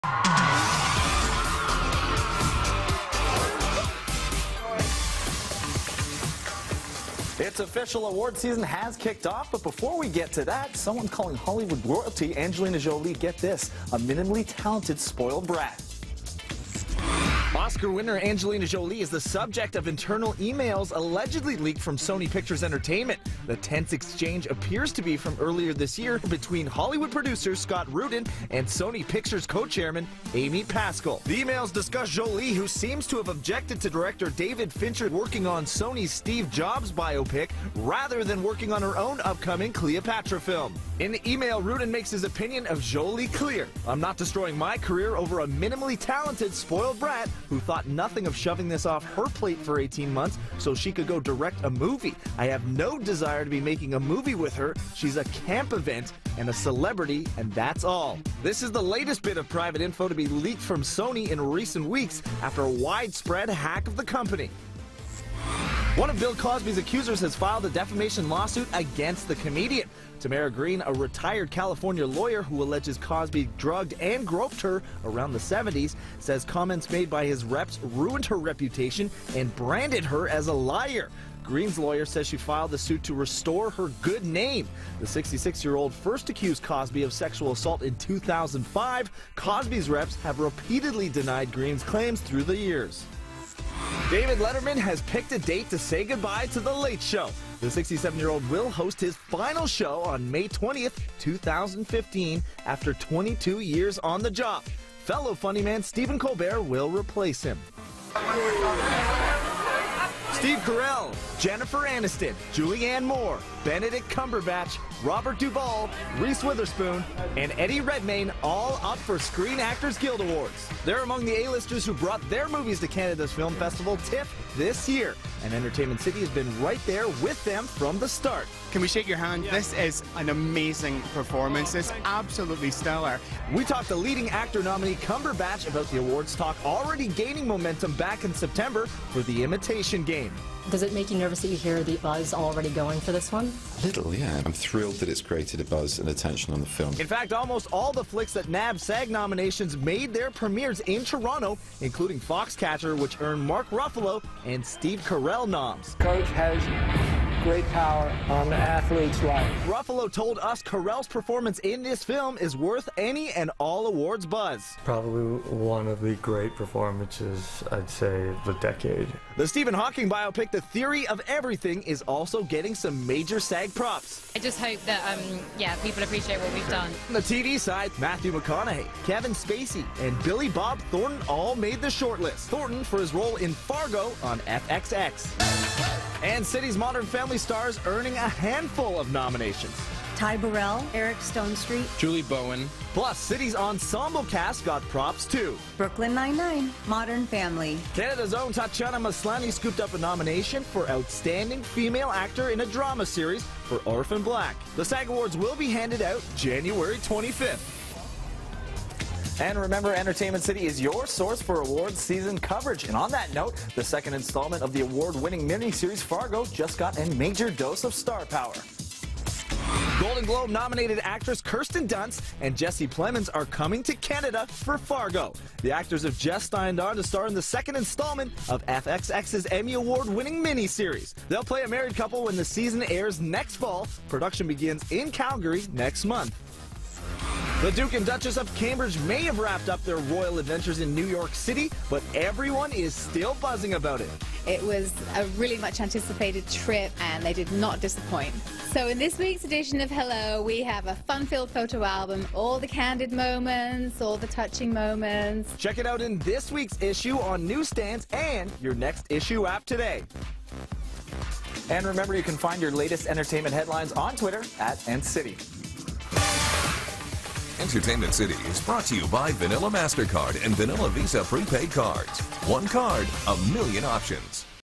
Entertainment City: Angelina Jolie called a spoiled brat
Say it isn’t so! Angelina Jolie gets called a spoiled brat. Brad Smith has the details in today’s Entertainment City.